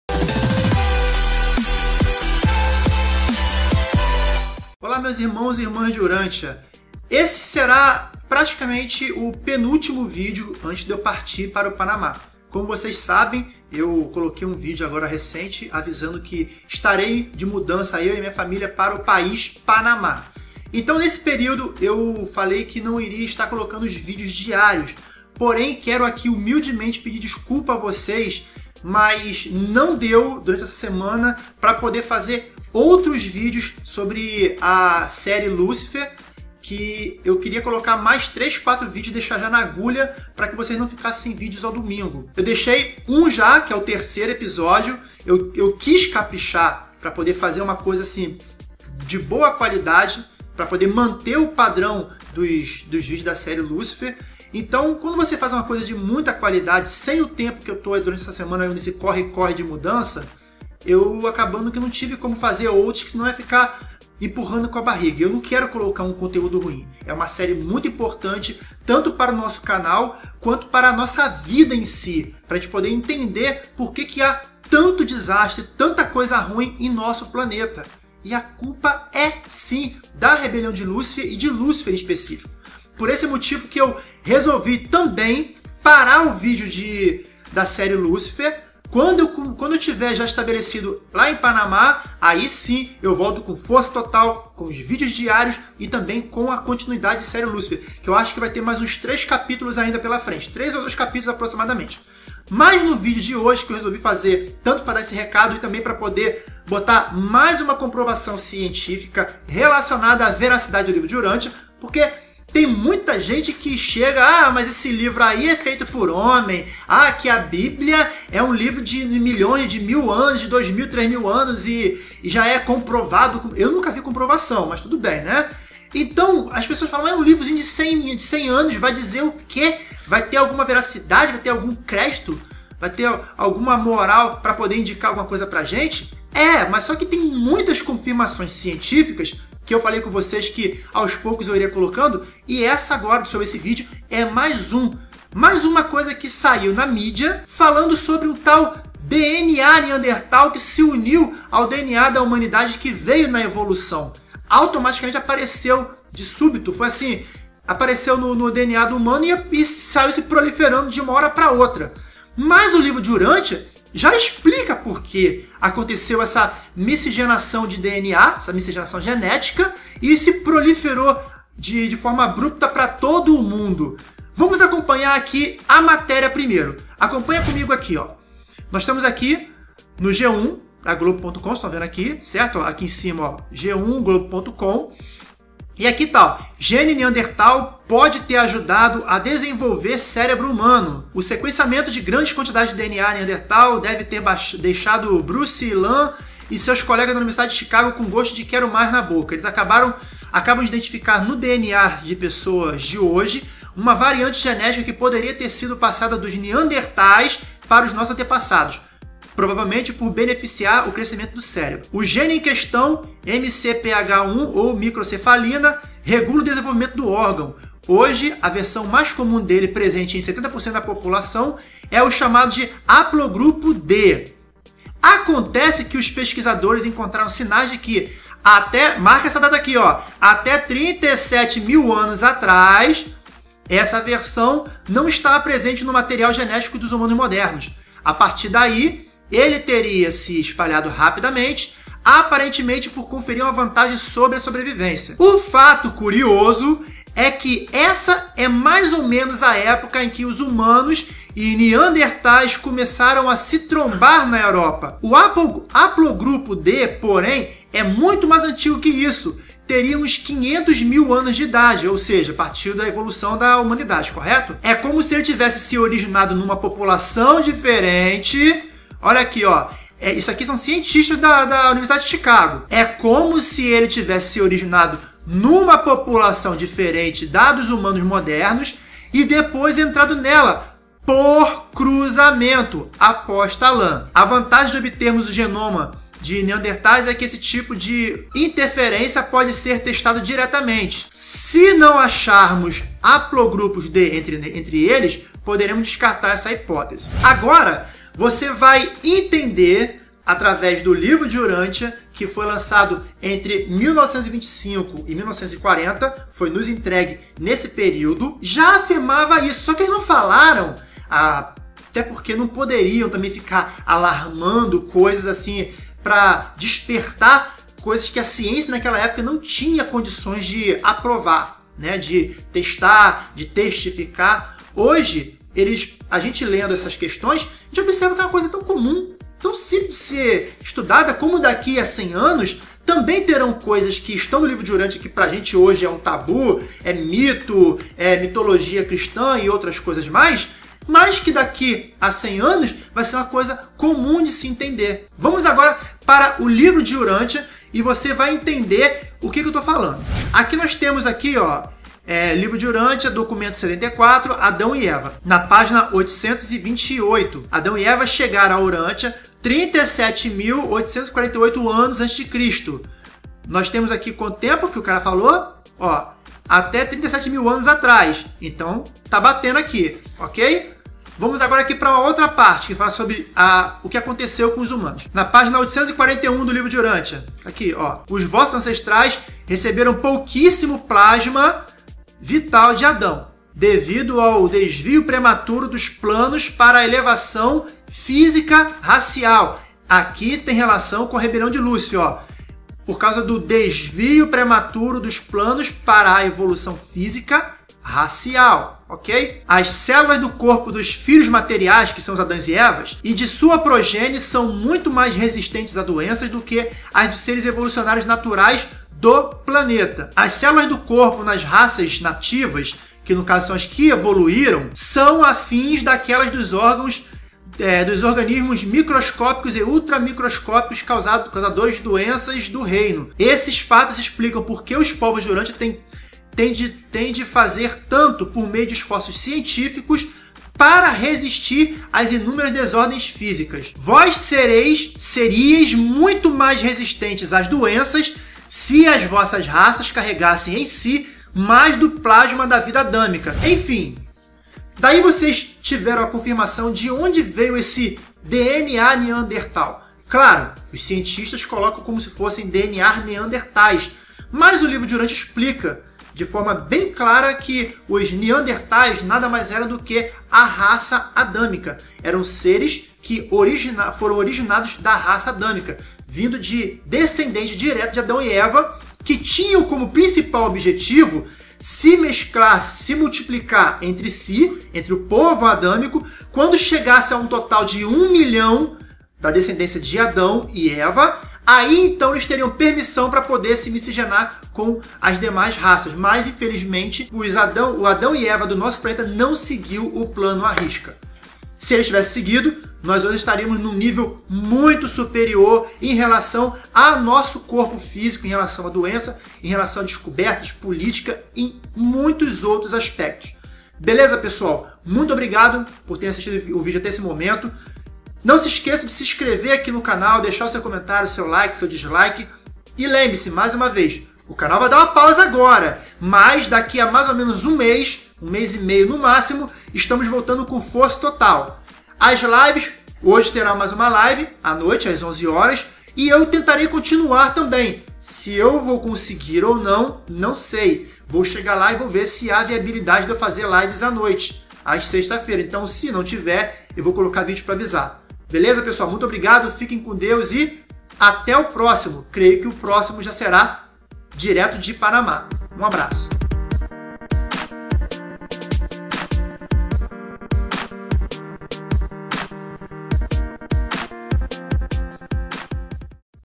🔵 DNA NEANDERTAL 🔵 COMPROVAÇÃO CIENTÍFICA de que o LIVRO DE URANTIA é VERÍDICO
Mais uma comprovação científica da veracidade do Livro de Urantia, agora sobre um tal "DNA Neandertal" encontrado no genoma humano.